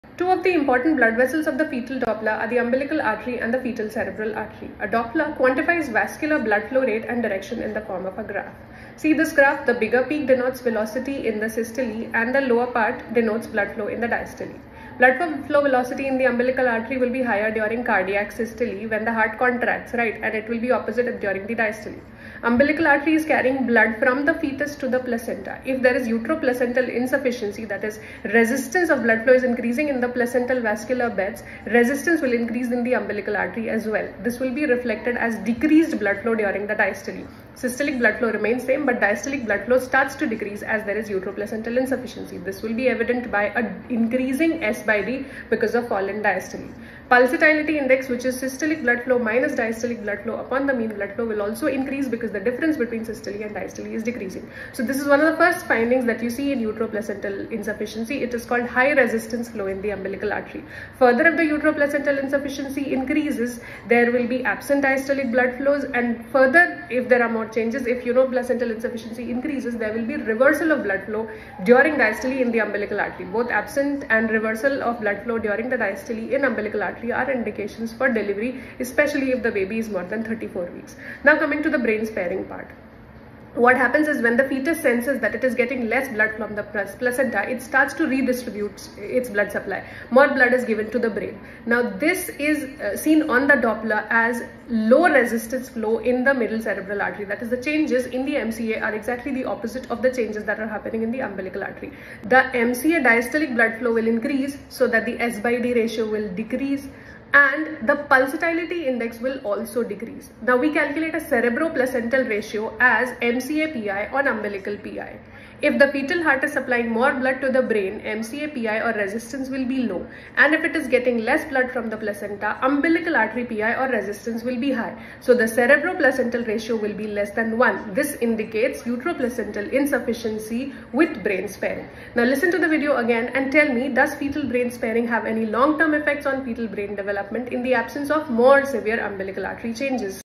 Fetal brain sparing effect
A video for OBGYNs and radiologists. Brain sparing does not lead to any brain developmental issues in the newborn - this is important while counselling a couple regarding fetal prognosis